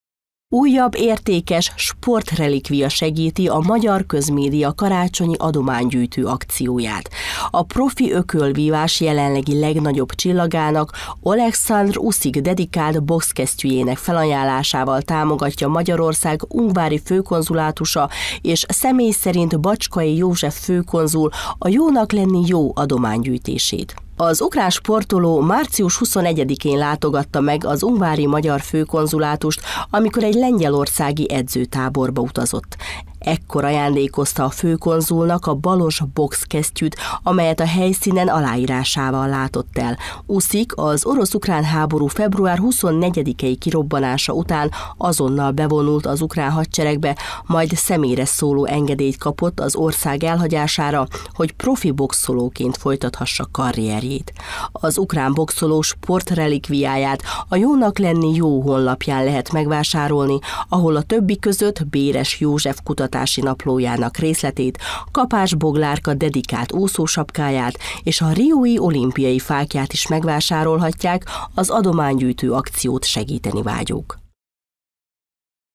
Tv21 Ungvár - Jónak lenni jó! – Uszik bokszkesztyűjét ajánlotta fel az Ungvári Magyar Főkonzulátus
Újabb értékes sportrelikvia segíti a magyar közmédia karácsonyi adománygyűjtő akcióját. A profi ökölvívás jelenlegi legnagyobb csillagának, Olekszandr Uszik dedikált bokszkesztyűjének felajánlásával támogatja Magyarország Ungvári Főkonzulátusa és Bacskai József főkonzul a Jónak lenni jó! adomány gyűjtését. Az ukrán sportoló március 21-én látogatta meg az Ungvári Magyar Főkonzulátust, amikor egy lengyelországi edzőtáborba utazott. Ekkor ajándékozta a főkonzulnak a balos bokszkesztyűt, amelyet a helyszínen aláírásával látott el. Uszik az orosz-ukrán háború február 24-i kirobbanása után azonnal bevonult az ukrán hadseregbe, majd személyre szóló engedélyt kapott az ország elhagyására, hogy profi bokszolóként folytathassa karrierjét. Az ukrán bokszoló sportrelikviáját a Jónak lenni jó! program honlapján lehet megvásárolni, ahol a többi között Béres József kutatási naplójának részletét, Kapás Boglárka dedikált úszósapkáját és a riói olimpiai fáklyát is megvásárolhatják az adománygyűjtő akciót segíteni vágyók.